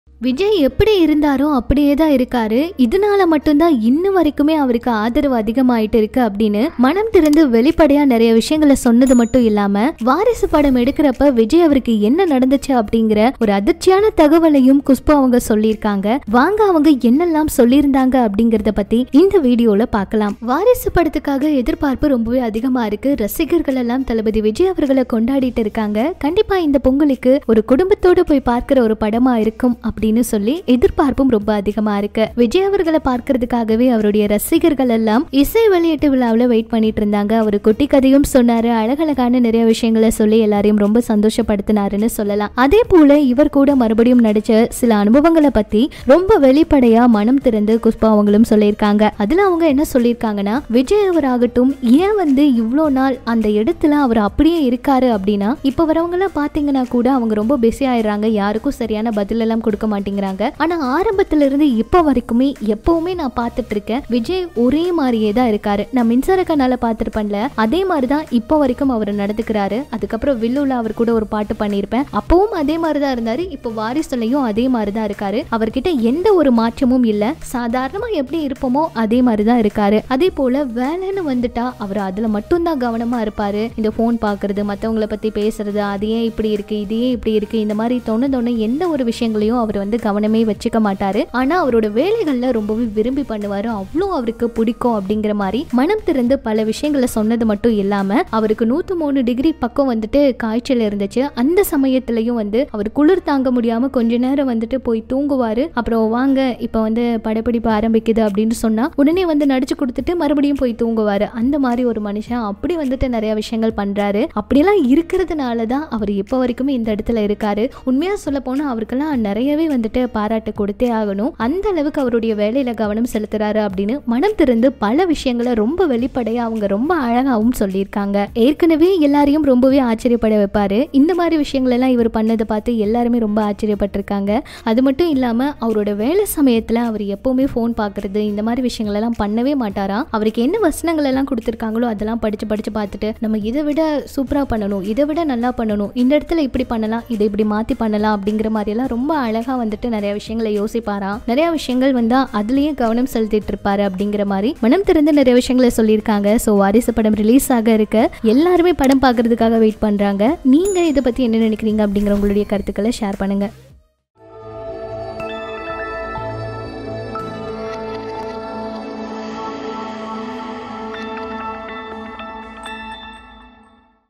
🔴 Exclusive: வாரிசு பற்றி பல உண்மைகளை சொன்ன குஷ்பு | Varisu | Kushboo Latest Interview about Vijay
🔴 Exclusive: வாரிசு பற்றி பல உண்மைகளை சொன்ன குஷ்பு | Varisu Movie | Kushboo Latest Interview about Vijay
While Thalapathy Vijay starrer Varisu is the most anticipated film with Khushbu in the lead role, Exclusive interview with actress Khushboo